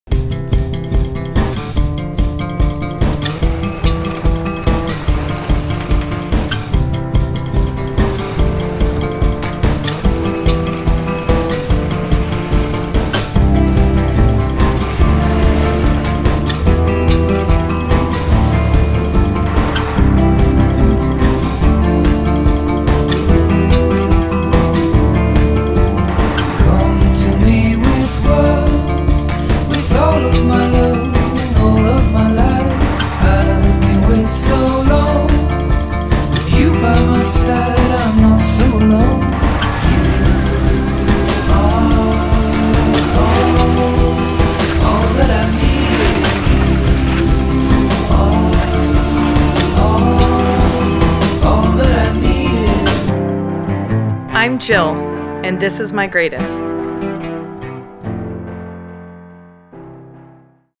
Jill Layfield - Find Your Greatest - Visit Utah
What’s Jill Layfield’s Greatest? A joyful family ski day in Utah. See more of this fun family story from mom and CEO Jill Layfield, then Find Your Greatest in Utah, home of The Greatest Snow on Earth®.
Plan your own Utah ski vacation and see what makes it a favorite among parents like Jill Layfield: